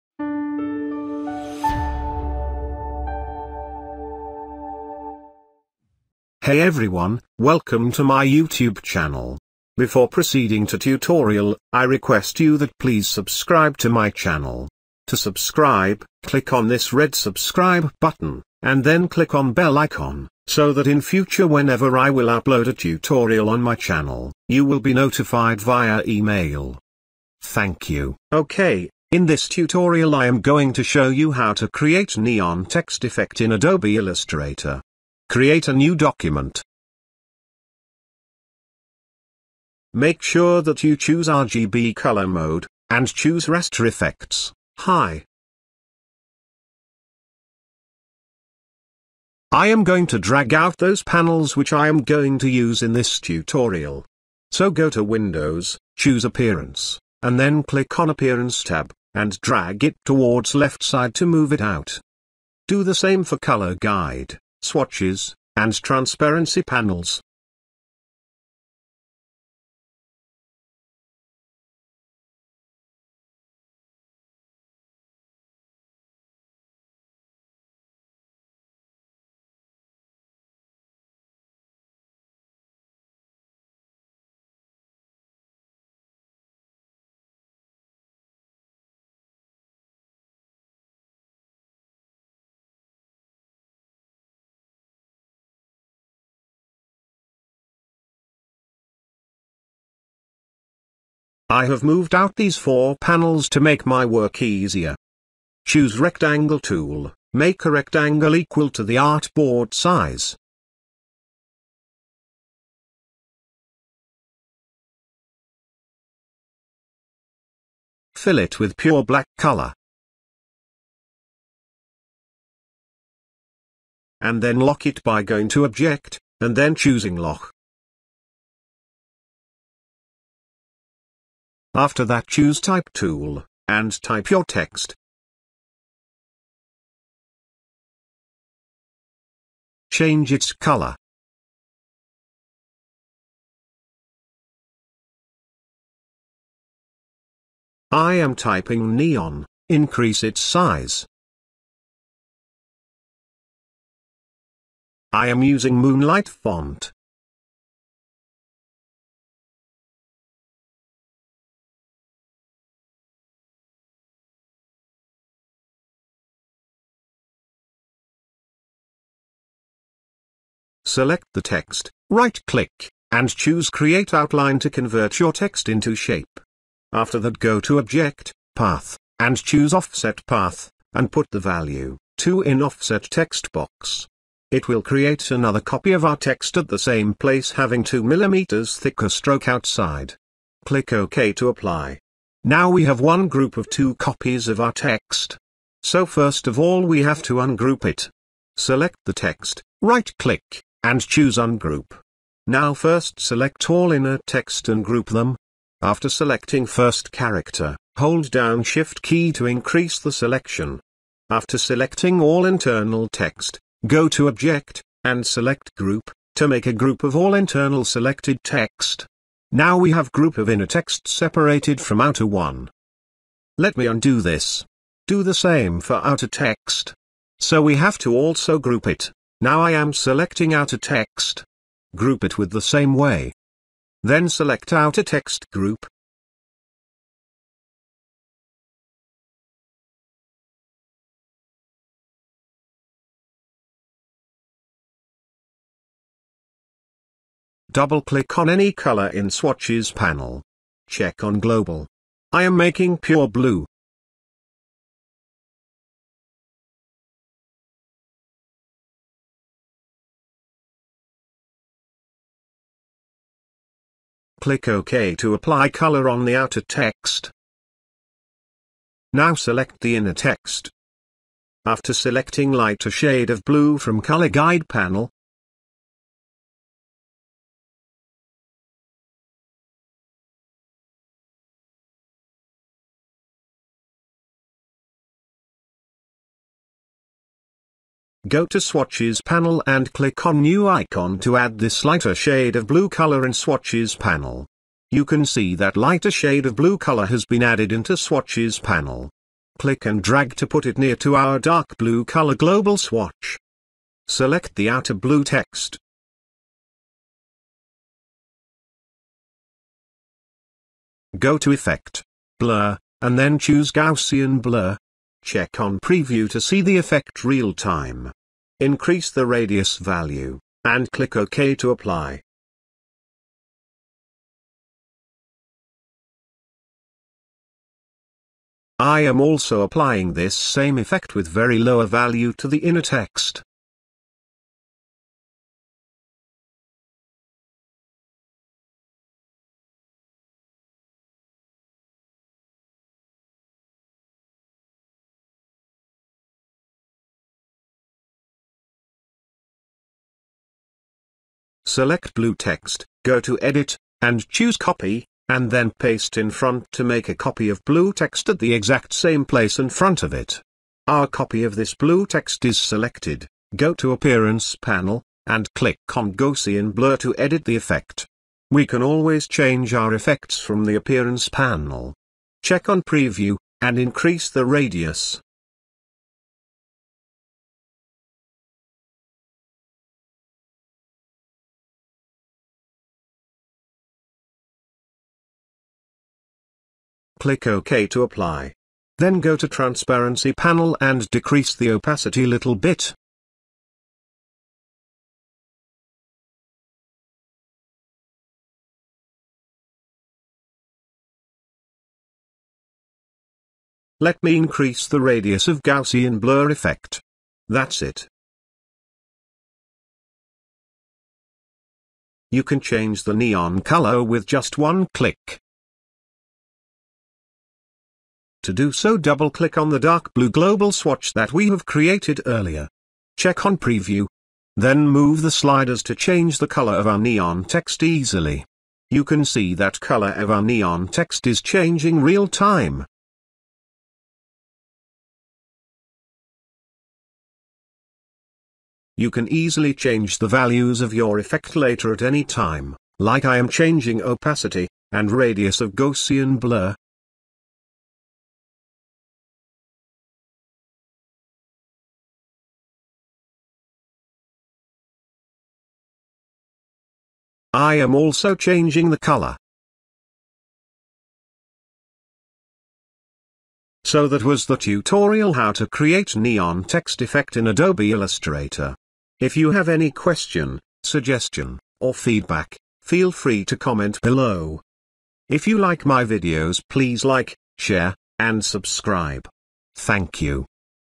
How to create Neon Text Effect in Adobe Illustrator
How to Create Neon Text Effect in Adobe Illustrator. I welcome you in another Adobe Illustrator Tutorial of Graphic Design Tutorials series. In this Adobe Illustrator Tutorial, we are going to make glowing means Neon Text in Adobe Illustrator.

In this tutorial we will use

Rectangle Tool
Text tool
Offset Path
Swatches Panel
Appearance Panel
Gaussian Blur Effect and
Transparency Panel

So, First of all make a rectangle of your art board size and fill it with full black color. Then type your text, convert it into shape by right clicking, and choosing create outlines.

Create a Global Swatches by Double clicking on any color in Swatches Panel, and marking check on Global check box.

After that make a thicker copy of that text by using offset path. Now we have to versions of text one is inner and other one is thinner as one group. So first ungroup it. Choose all inner text first and make its group. Similarly choose all outer text and group it.
After that apply Gaussian Blur on outer and inner text. Then take a copy of outer text using paste in front and increase its value from previous one using appearance panel.

That’s it. So that was the Adobe Illustrator Tutorial for creating Neon Text in Illustrator, I hope that you have enjoyed this Adobe Illustrator Tutorial.